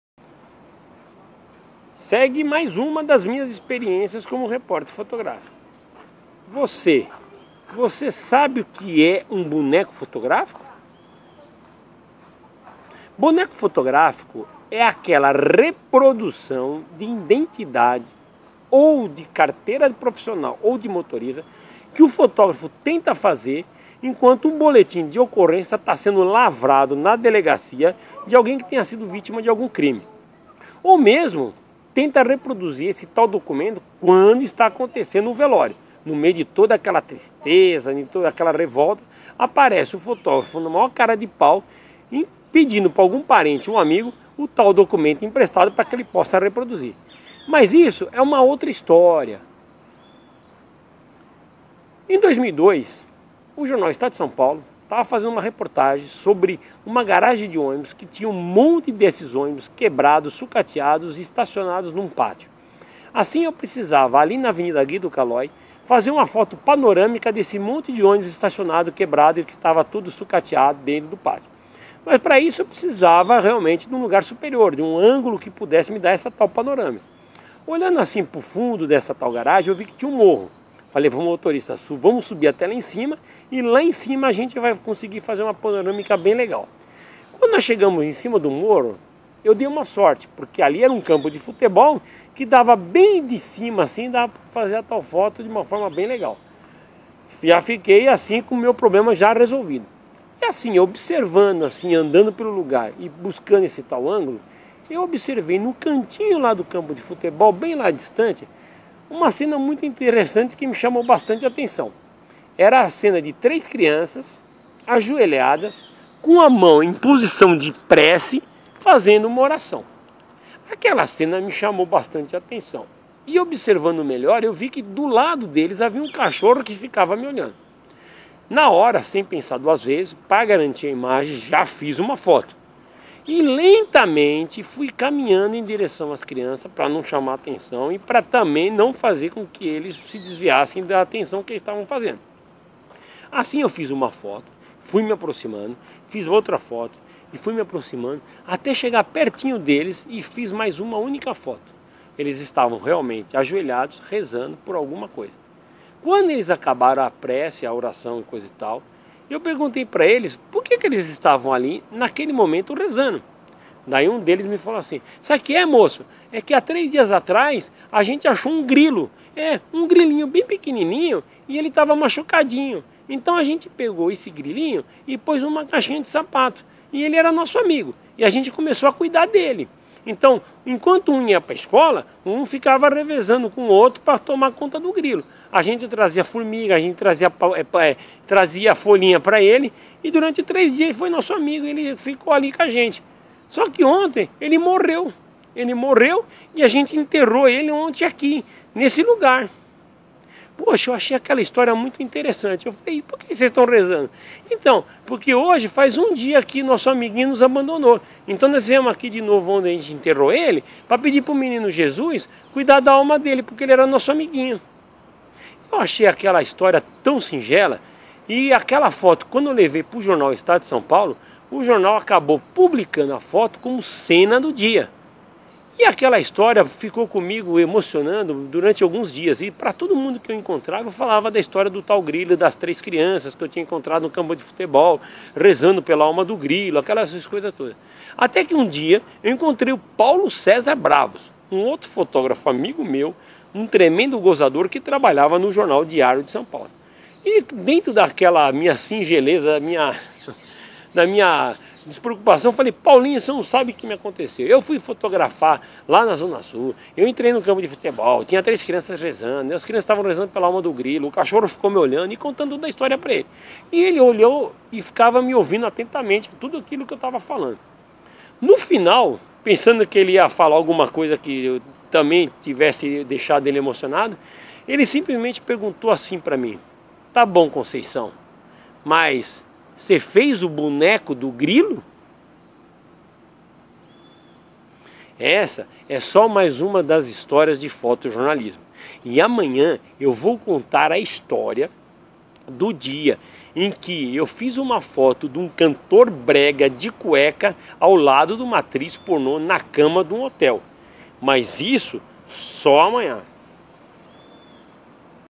José Luis da Conceição " Oração pela alma do Grilo"
Nesta aventura, narro o dia em que eu encontrei com a cena de três crianças e um cachorro rezando pela alma do grilo.